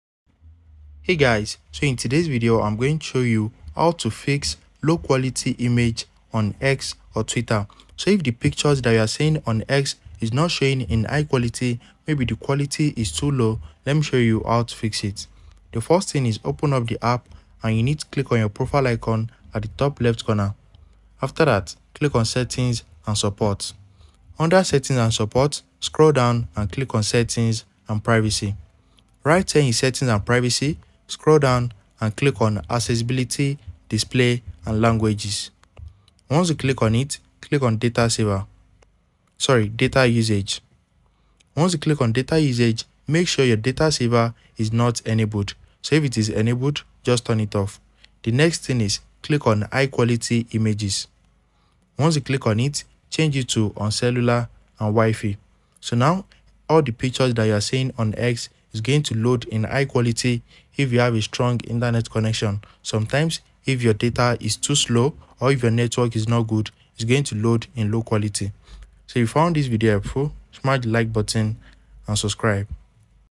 How to FIX Low Quality Images on X (Twitter)
Frustrated with low-quality images on X (formerly Twitter)? Learn how to troubleshoot and fix common issues that may be causing blurry or pixelated images. This video will provide solutions to improve image quality and enhance your X experience.